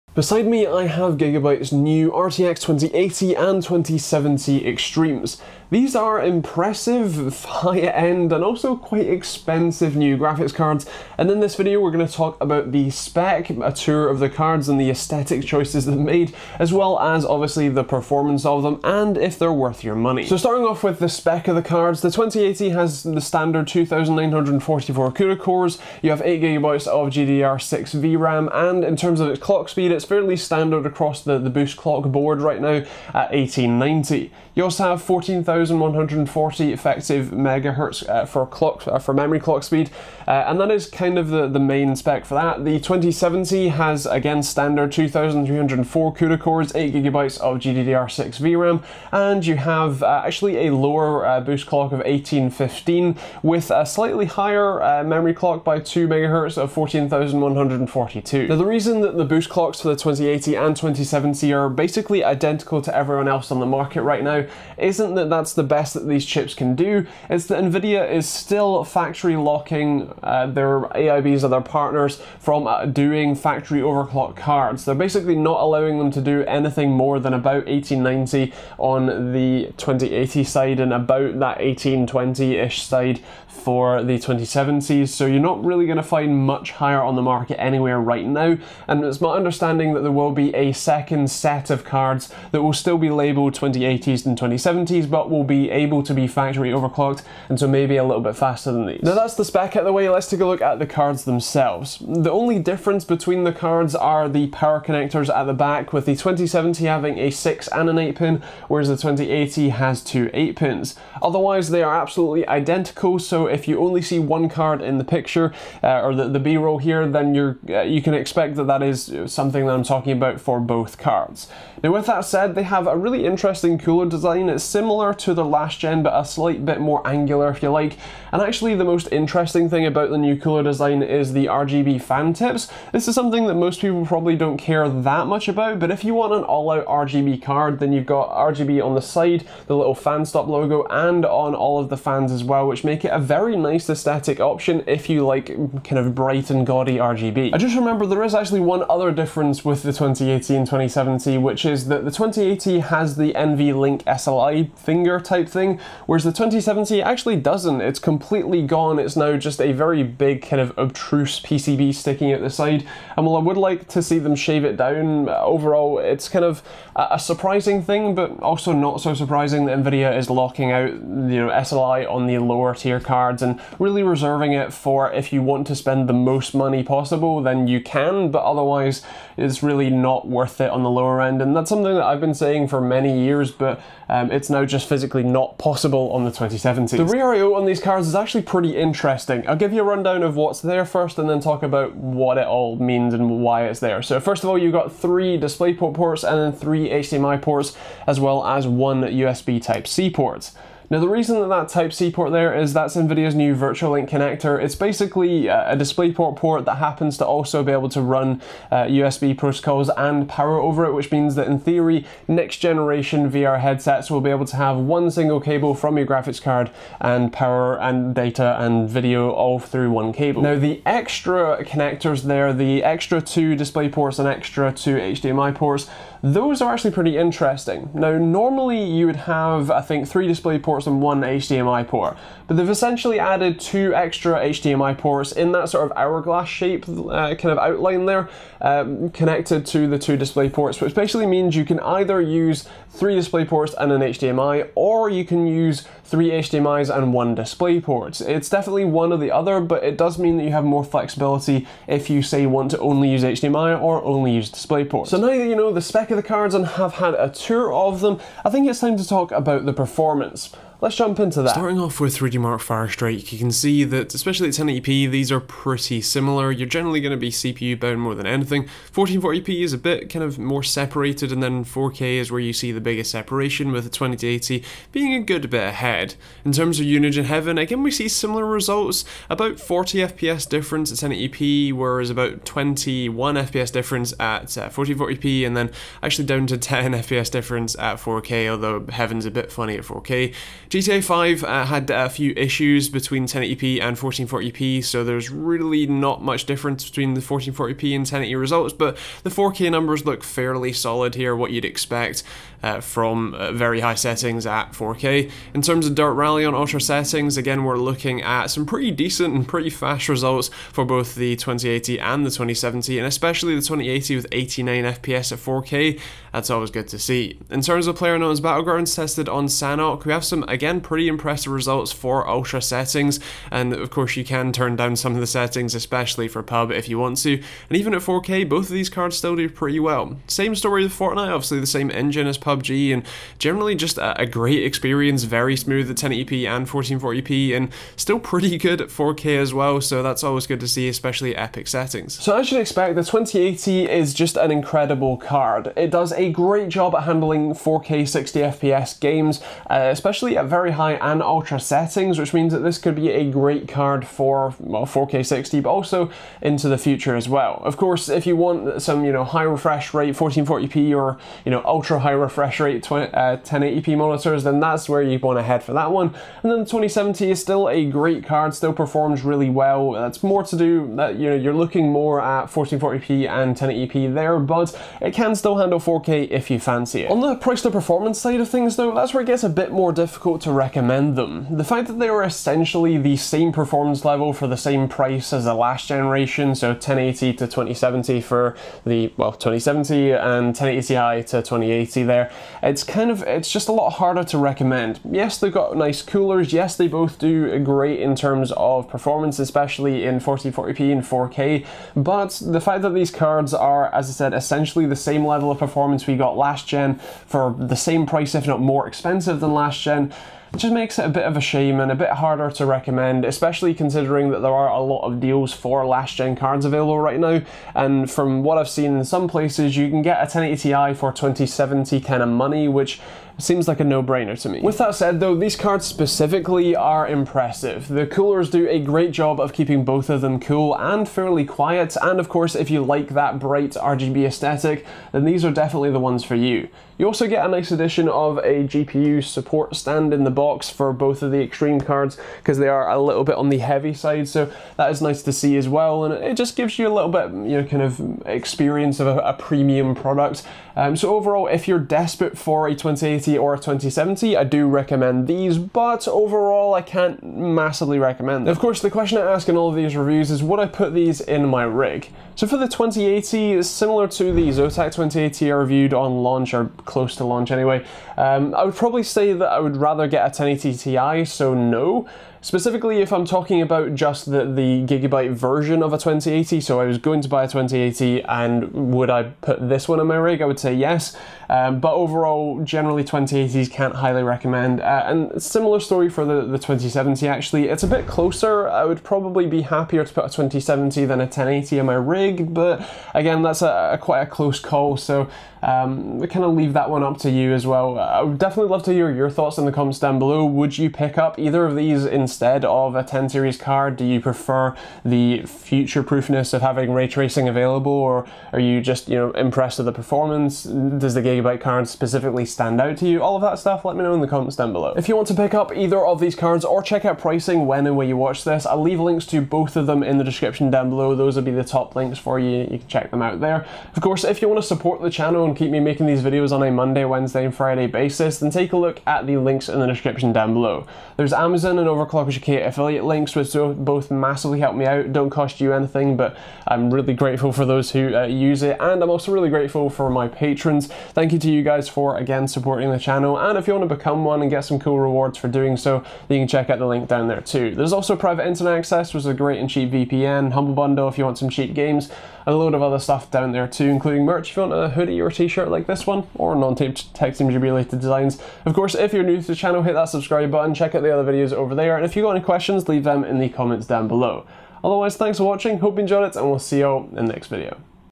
Gigabyte RTX 2080 & RTX 2070 XTREME Review
These new cards from Gigabyte have some great power, and some rather bright RGB lights - but are they actually worth your money, and a spot in your gaming PC? Let's take a look.
Products shown provided by: Gigabyte